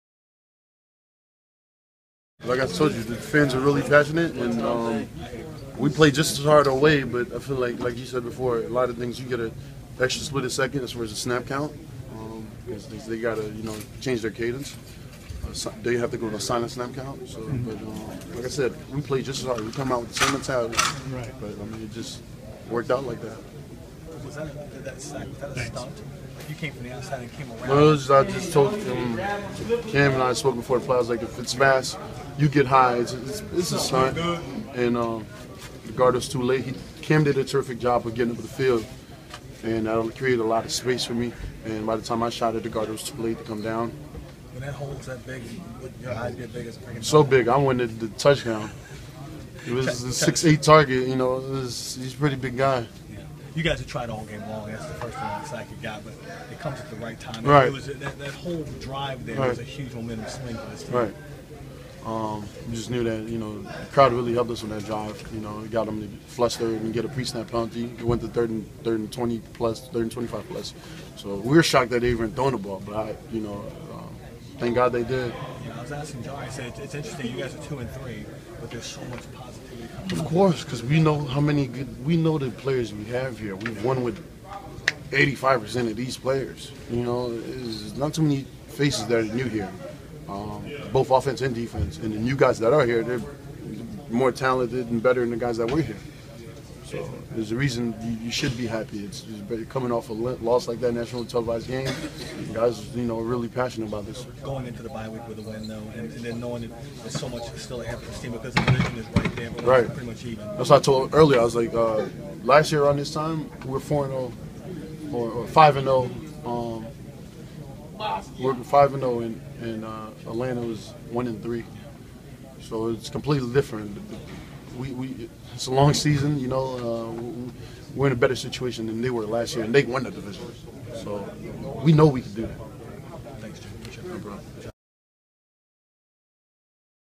Junior Galette talks about his game-changing play Sunday | Video
New Orleans Saints linebacker Junior Galette talks about his game-changing play Sunday in the Saints 37-31 overtime victory against the Buccaneers.